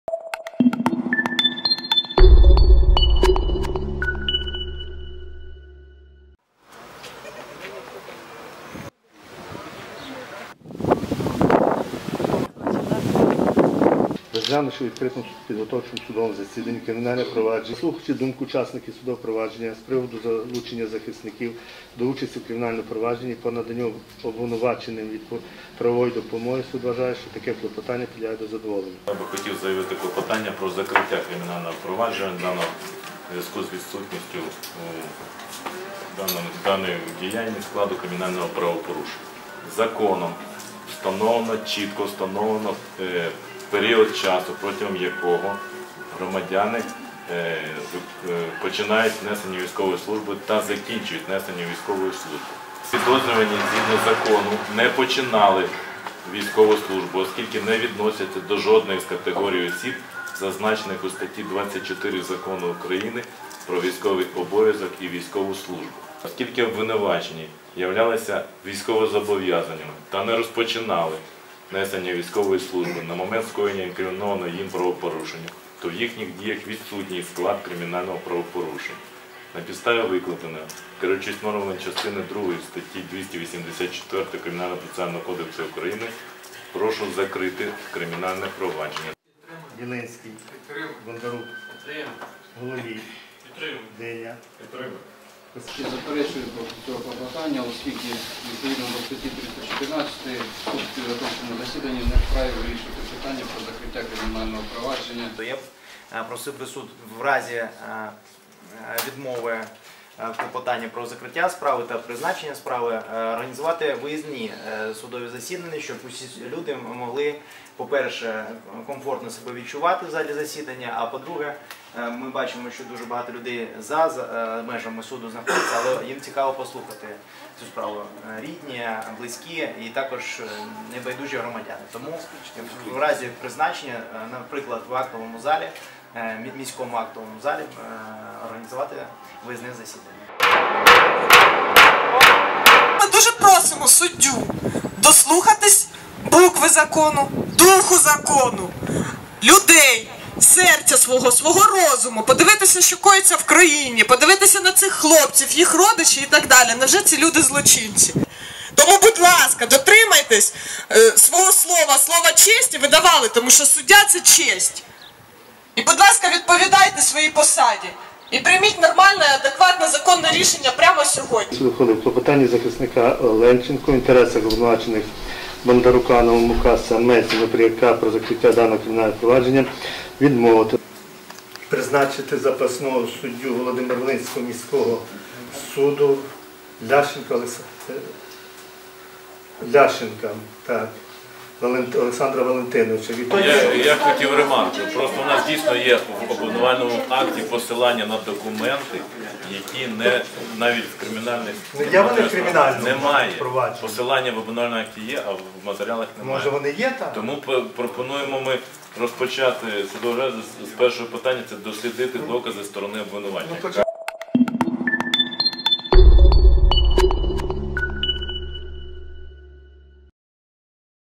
Кримінальну справу проти бійців 14-ї бригади розглядатимуть по суті
Володимир-Волинський міськрайонний суд дійшов до етапу розгляду кримінальної справи по суті проти бійців 14-ї (раніше 51-ї) механізованої бригади. Про це стало відомо на останньому підготовчому засіданні, що відбулося у п’ятницю, 10 липня.